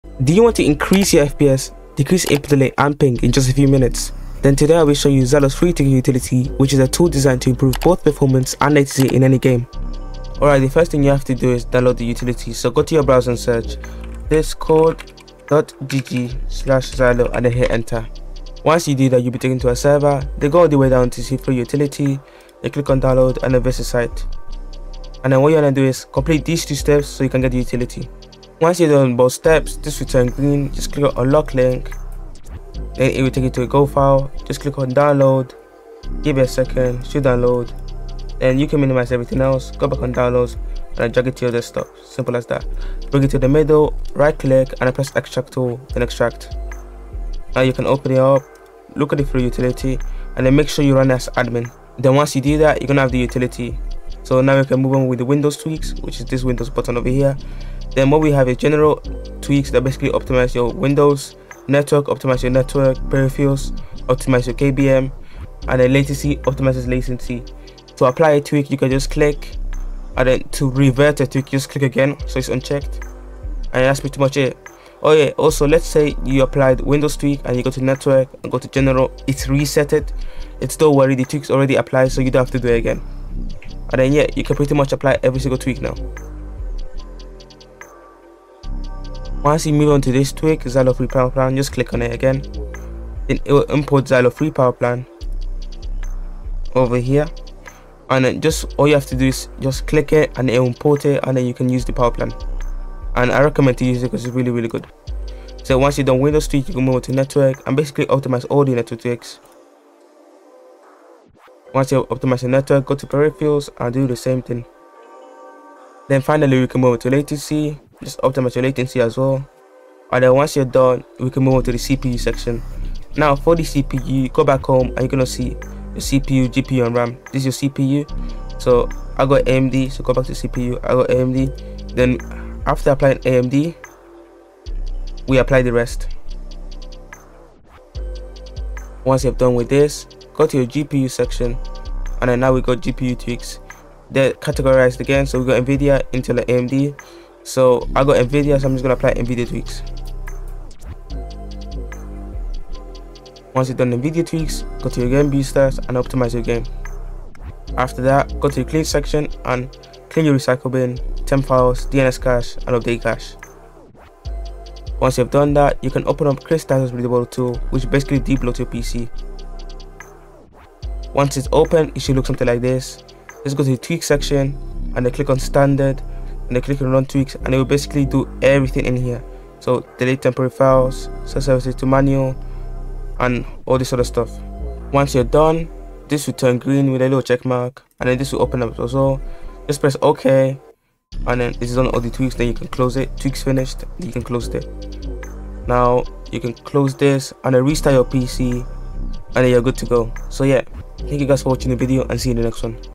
ULTIMATE FPS Boost Tool 🔧(Boost FPS, Lower Input Delay & Latency)
Timestamps:
0:00 - 0:11 Intro
0:12 - 0:52 How to download
0:53- 3:26 How to use the utility
3:35 - 3:52 Outro